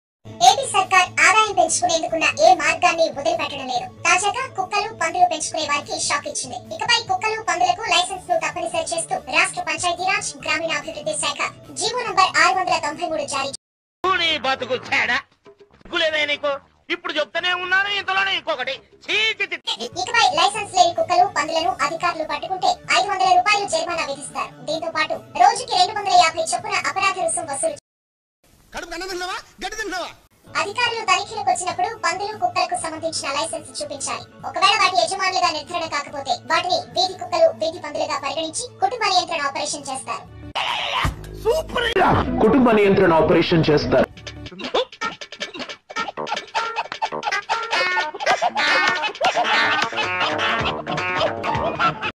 వీధి కుక్కలను కూడా వదలని ఏపీ సర్కారు ||trolling
కుక్కలకు పందులకు లైసెన్స్ తప్పనిసరి చేస్తూ ప్రభుత్వం  జీవో జారీ చేసింది